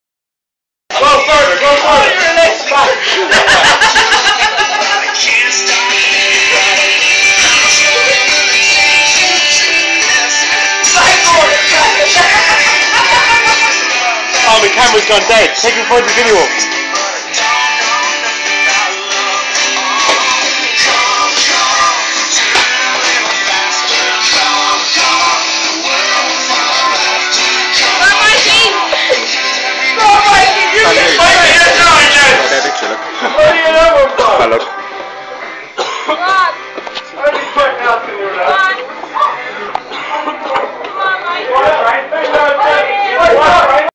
The jig!
Michael Glendon doin a jig in glendons bar!